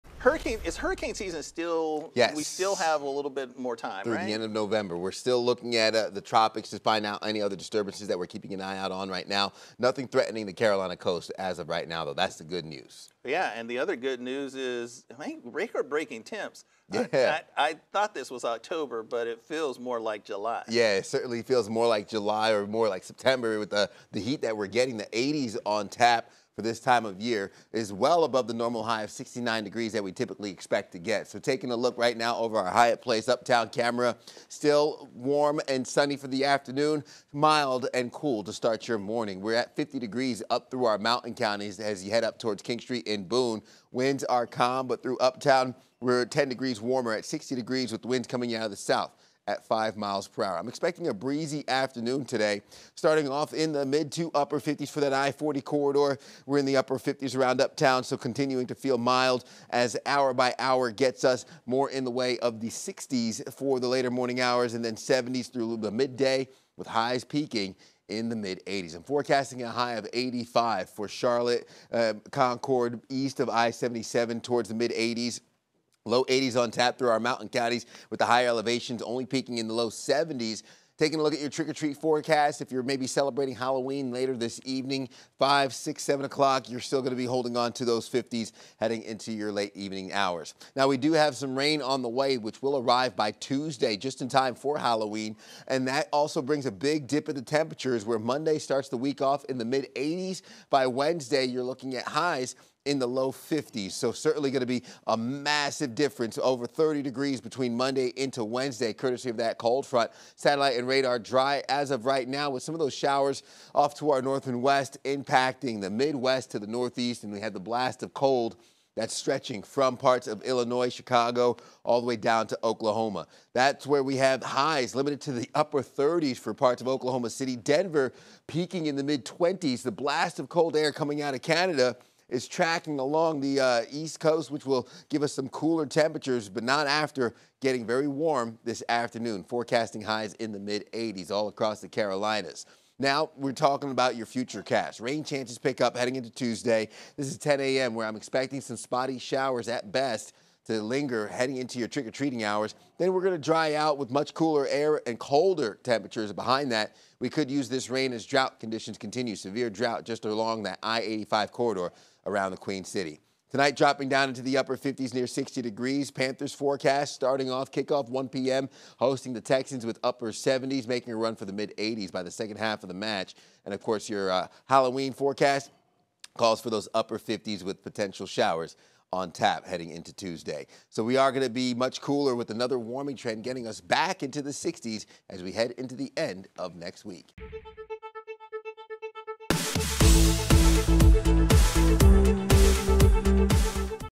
Sunday Morning Forecast | October 29, 2023
Charlotte hit 84 degrees on Saturday-- *not* a record (the record was 87). However, Sunday’s record will likely be broken (83 degrees). We're forecasting 86. Staying dry for the Panthers game!